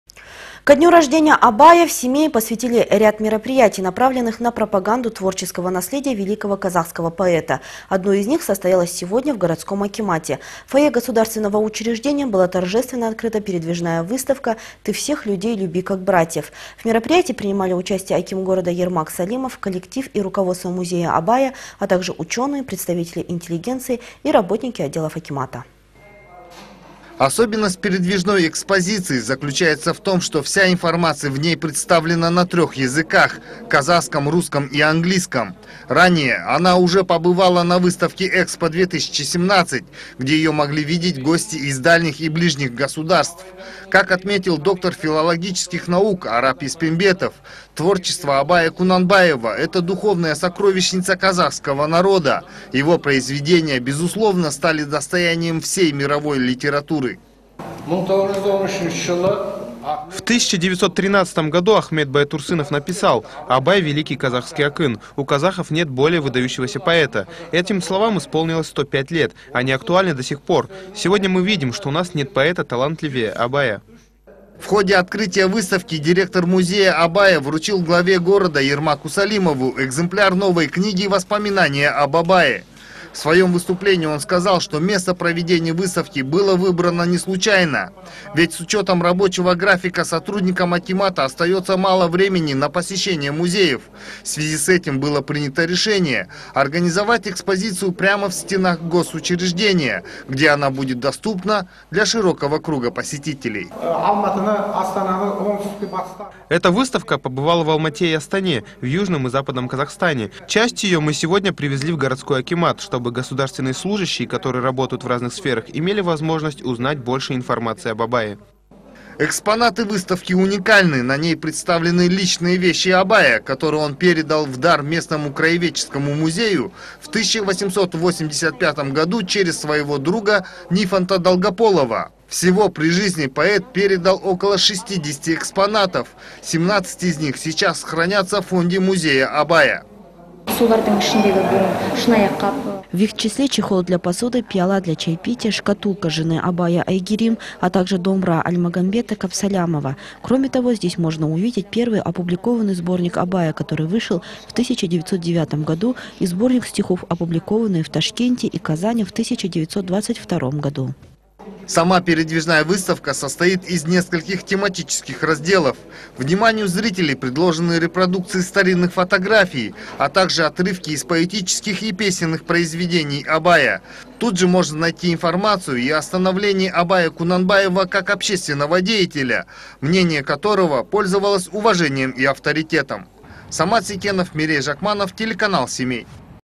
«ТЫ ВСЕХ ЛЮДЕЙ ЛЮБИ КАК БРАТЬЕВ».
Ко дню рождения Абая в Семее посвятили ряд мероприятий, направленных на пропаганду творческого наследия великого казахского поэта. Одно из них состоялось в городском акимате. В мероприятии принимали участие аким города Ермак Салимов, коллектив и руководство музея Абая, а также ученые, представители интеллигенции и работники отделов акимата.
САМАТ СЕКЕНОВ